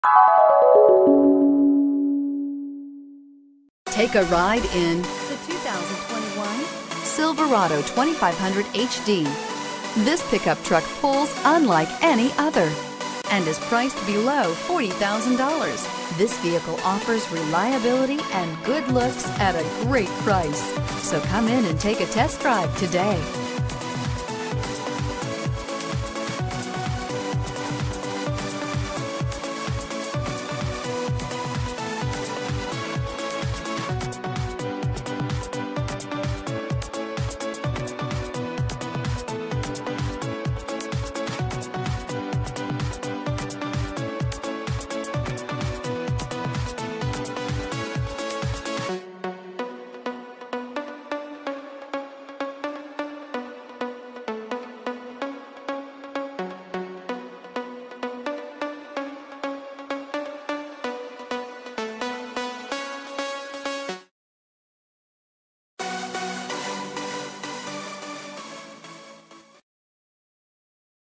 2021 Chevrolet Silverado 2500HD Mooresville NC TR82032
2021 Chevrolet Silverado 2500HD Work Truck

Pick up this  2021 Chevrolet Silverado 2500HD, available today at Randy Marion Chevrolet Buick Cadillac. This could be the one you've been searching for.